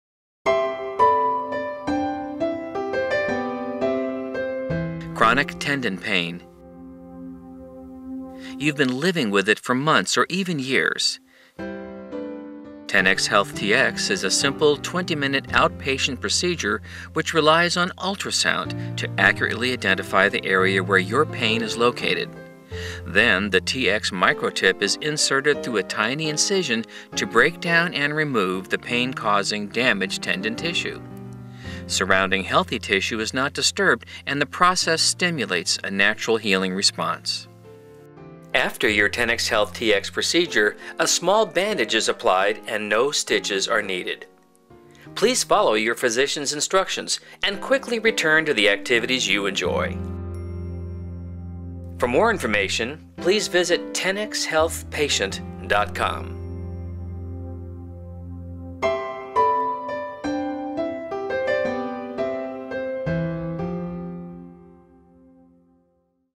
Tenex Procedure- Animation
RestorePDX utilizes Tenex to help relieve the tendon pain, tendonitis, that limits your life. Using ultrasound guidance, Tenex, removes the damaged tissue that is causing the pain.